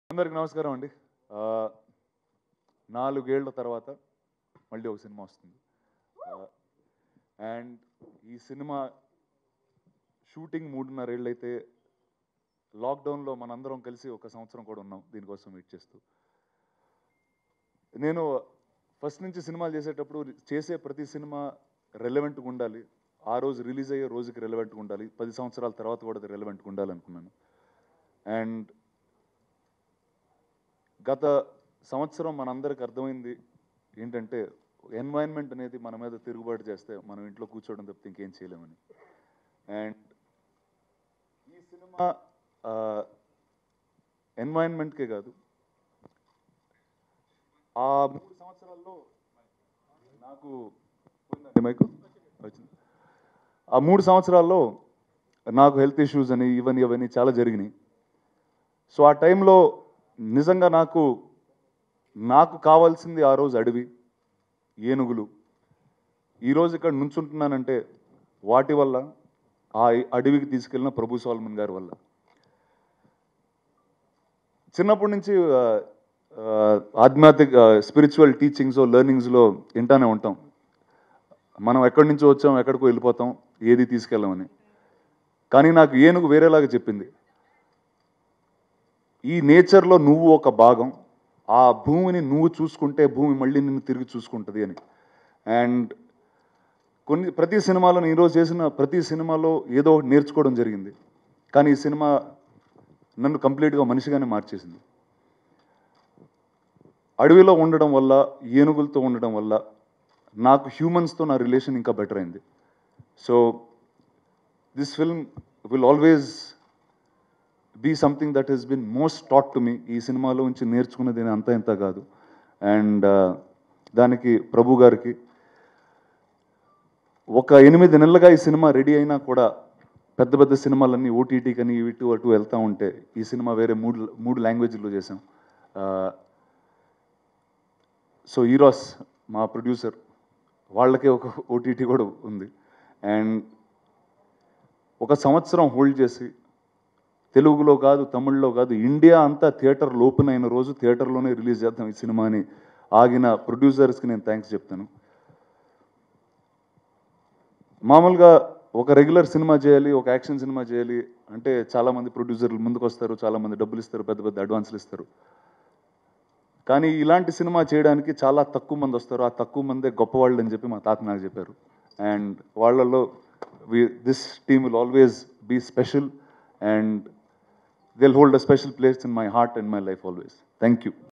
Rana Daggubati Shocking Words About His Health Condition | Aranya Trailer Launch | Andhra Life Tv
Watch Here :Rana Daggubati Shocking Words About His Health Condition